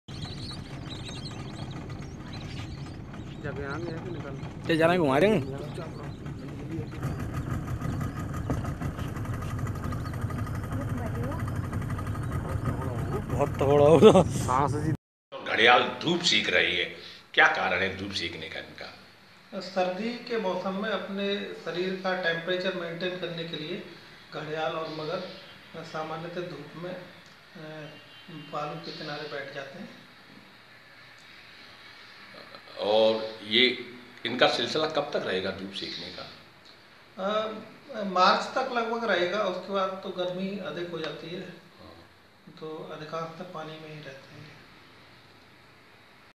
चंबल में धूप सेक रहे घड़ियाल और मगर, पशु प्रेमी पर्यटकों का आवागमन शुरू
आगरा : बाह और पिनाहट कस्बा के अंतर्गत चंबल नदी के बीहड़ों में इस समय घड़ियाल और मगरमच्छ आसानी से दिखाई दे रहे हैं. दूर-दूर से लोग यह अद्भुत नजारा देखने के लिए चंबल के घाटों पर पहुंच रहे हैं.